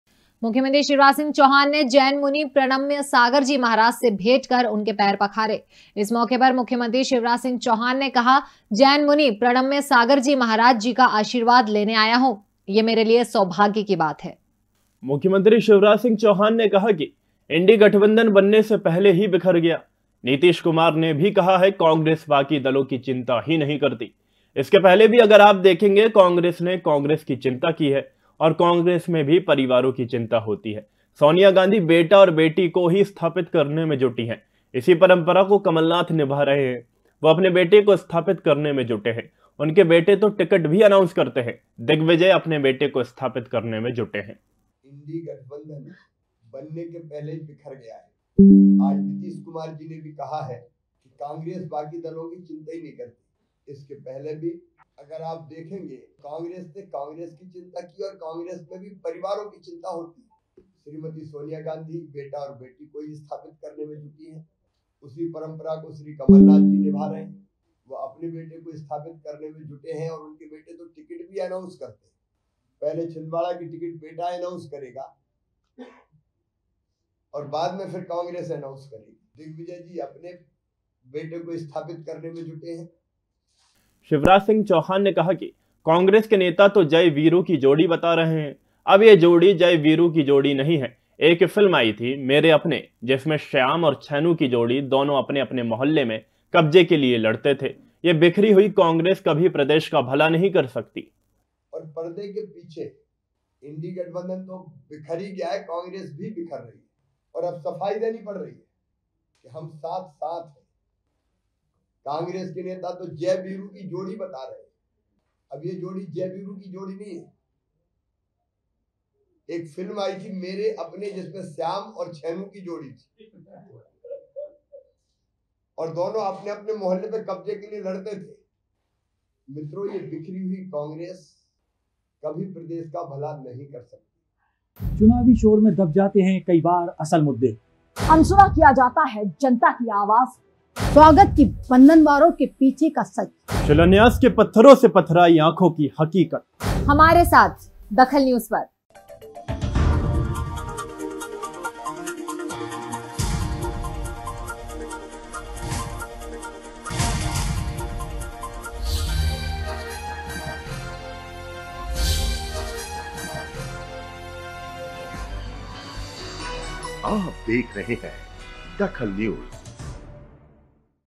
DAKHAL NEWS कमलनाथ और दिग्विजय श्याम और छेनू जैसे
कमलनाथ और दिग्विजय श्याम और छेनू जैसे
इंडी गठबंधन बनने से पहले ही बिखर गया है